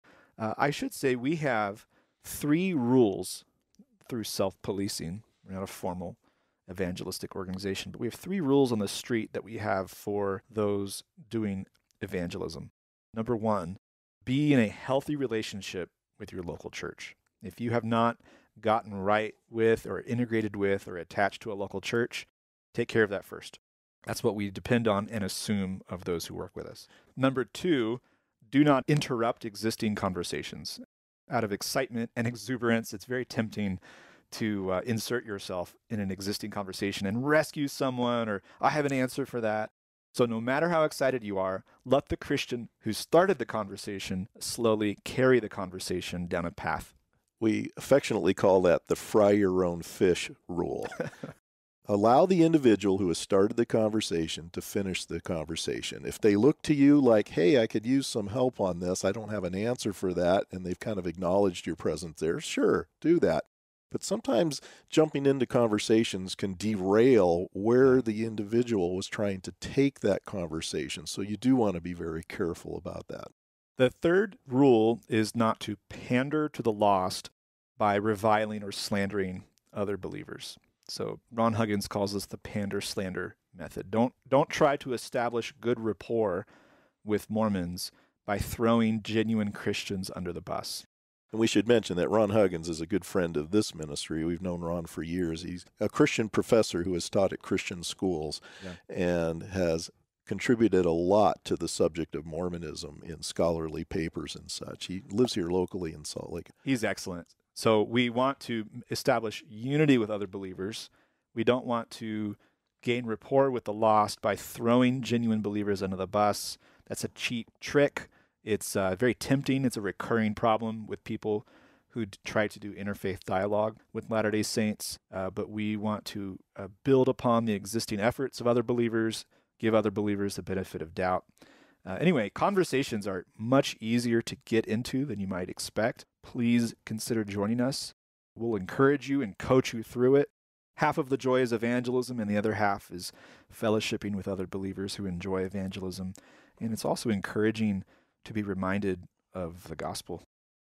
Three Rules for Street Evangelism (Utah Evangelists)
Rule #1: Be in a healthy relationship with a local church.

Rule #2: Don't interrupt existing conversations out of excitement and exuberance, but allow the person who started the conversation to carry it forward.

Rule #3: Don't pander to the lost by reviling or slandering other believers.